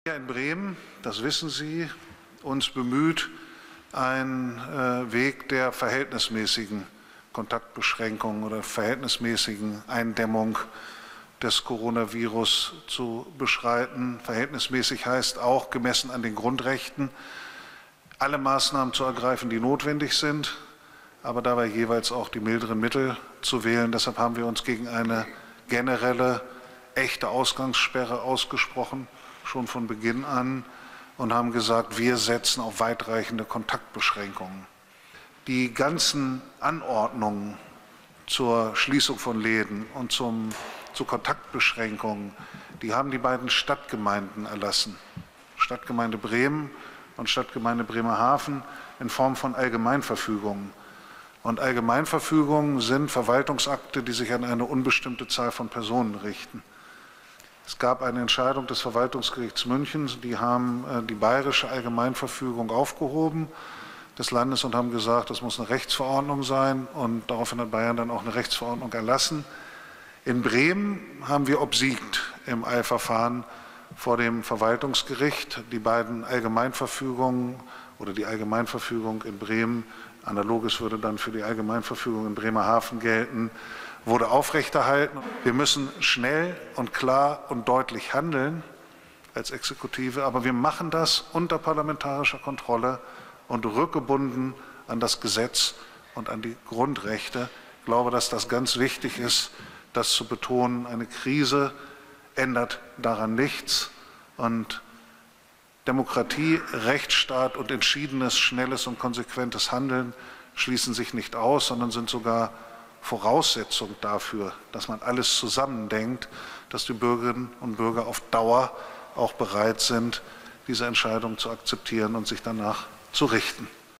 Andreas Bovenschulte zu den Kontaktbeschränkungen in Bremen in der Coronakrise am 27.03.20
Andreas Bovenschulte (SPD), der Bürgermeister der Stadt Bremen, erklärt in einem Pressestatement, wie sich die Kontaktbeschränkung in Bremen im Detail gestaltet und welche Voraussetzungen von politischer und gesellschaftlicher Seite gelten müssen, damit die Maßnahmen eingehalten werden können.